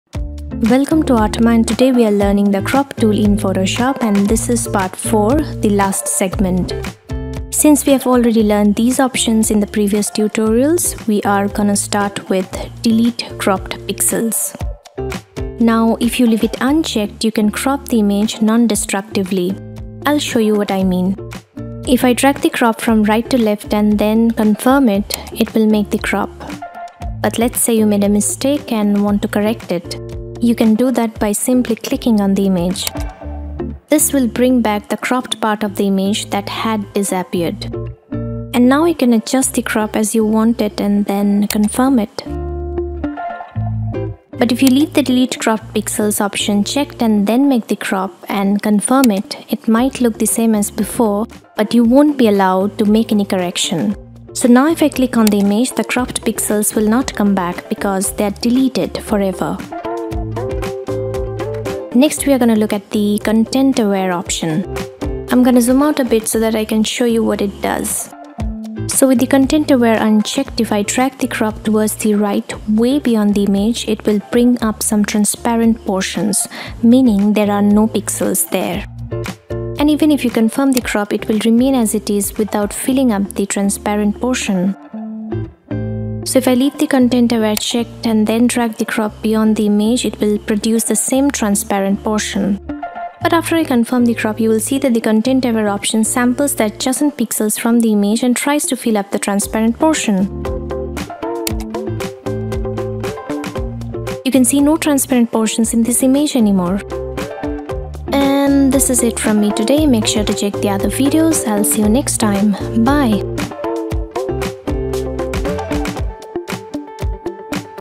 ✔ Crop Tool Part- 4 | Photoshop Tutorial | Artose
Cropping an image is very easy. Moreover, there are various other options that come with the crop tool that make for your specific cropping needs. The crop tool can also correct an unaligned horizon or other edges in an image. You can adjust your crop in many ways that let you move rotate or scale it so that you can target a desired spot in the image. You will be fascinated how the crop tool can help you correct a wrongly photographed composition. I hope you enjoy this tutorial.

In this Photoshop Tutorial we will learn the Crop Tool covering the following topics :
Delete Cropped Pixels 0:12
Content Aware Fill 1:00

LINKS